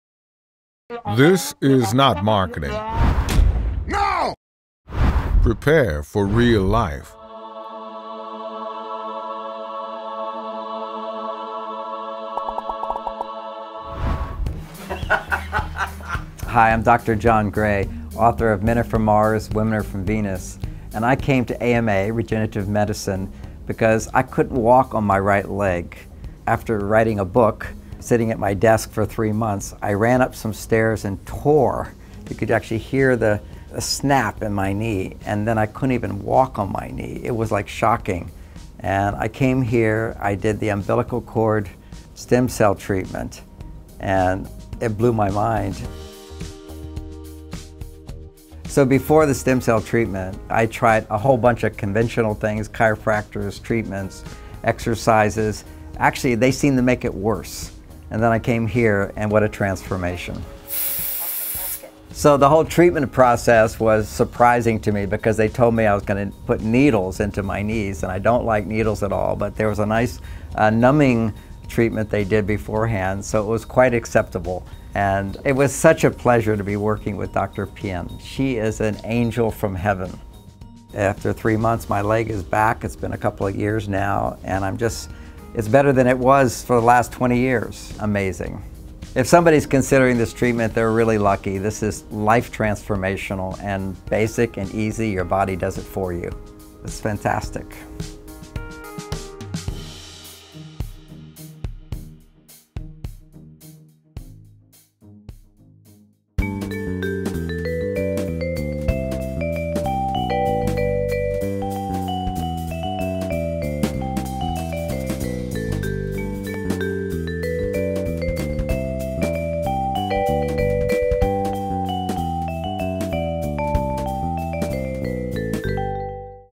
Dr. John Gray - My Experience with Stem Cell Knee Injections
Hi I’m Dr. John Gray author of Men Are from Mars, Women Are from Venus. I came to AMA Regenerative Medicine because I couldn’t walk on my right leg. After writing a book, sitting at my desk for 3 months, I ran up some stairs and tore, you could actually hear a snap in my knee and then I couldn’t even walk on my knee. It was like shocking.

I came here to AMA Regenerative Medicine and I did the umbilical cord stem cell treatment and it blew my mind.

Before the stem cell treatment I tried a whole bunch of conventional things. Chiropractor treatments, exercises, actually they seemed to make it worse. Then I came here for the stem cell injections and what a transformation.

The whole treatment process was surprising to me because they told me they were going to put needles into my knees and I don’t like needles at all but there was a nice numbing treatment  that they did beforehand. So it was quite acceptable and it was such a pleasure to be working with Dr. Pien. She is an angel from heaven.

After 3 months my leg is back. It’s been a couple of years now and it’s better than it was for the last 20 years. Amazing!

If somebody is considering stem cell treatments they are really lucky. This is life transformational, basic and easy. Your body does it for you. It’s fantastic!

AMA Regenerative Medicine & Skincare
Alice Pien, MD
Asher Milgrom, PhD

Irvine / Newport Beach
2302 Martin St. Suite 400

Beverly Hills / Los Angeles
6310 San Vicente Blvd. Suite 285